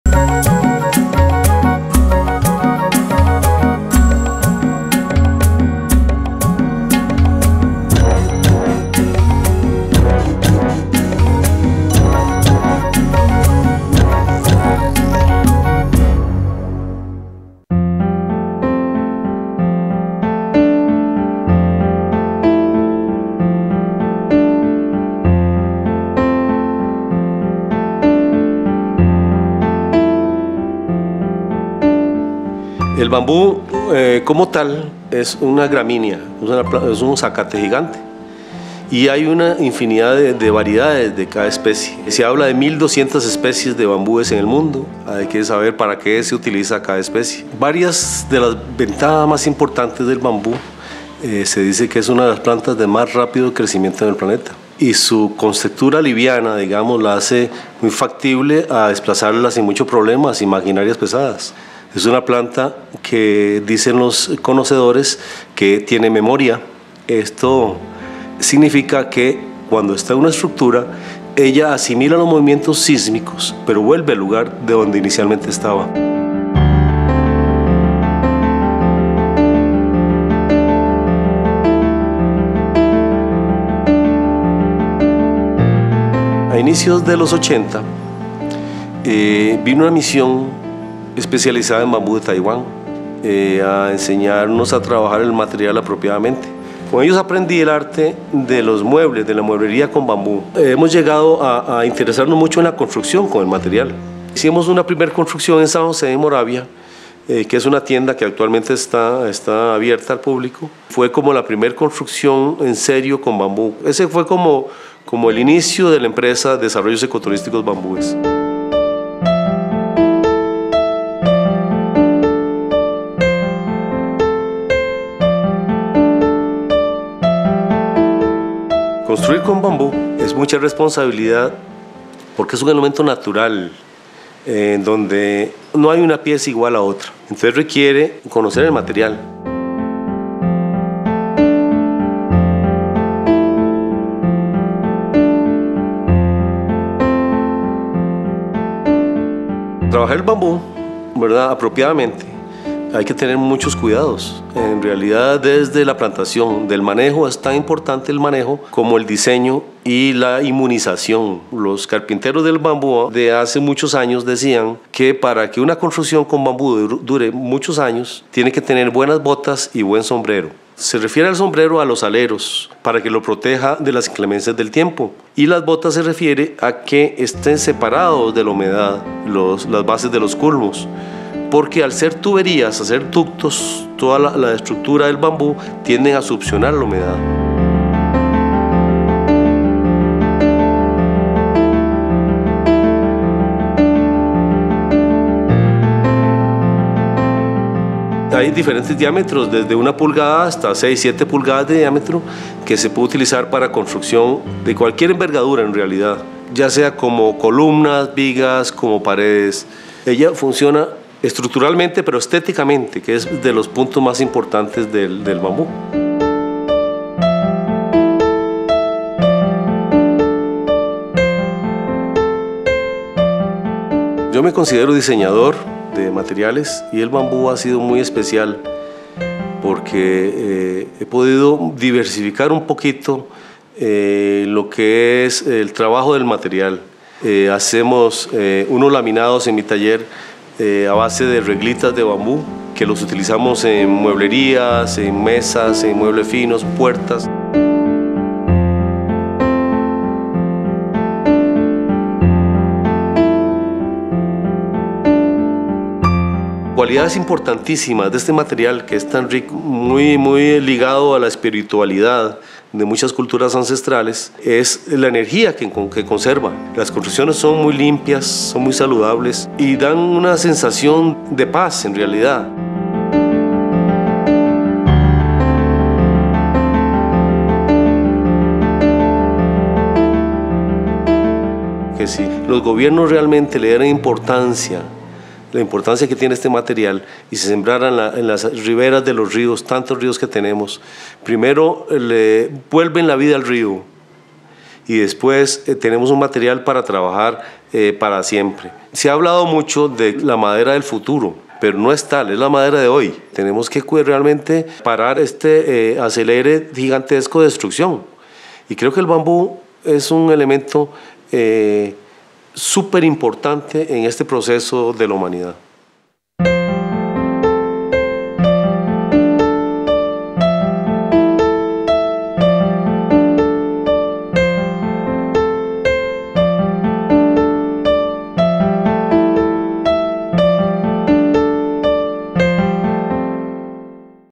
MORAVIA Arquitectura Bambú
Desarrollos Ecoturísrticos Bambúes es un proyecto de arquituctura sostenible. Aplica las mutiples posibilidades del Bambú en la construcción, desde su uso en la parte estructural, hasta los acabados.
El Bambú, en palabras de ellos mismos, no es la madera del futuro es la madera de hoy.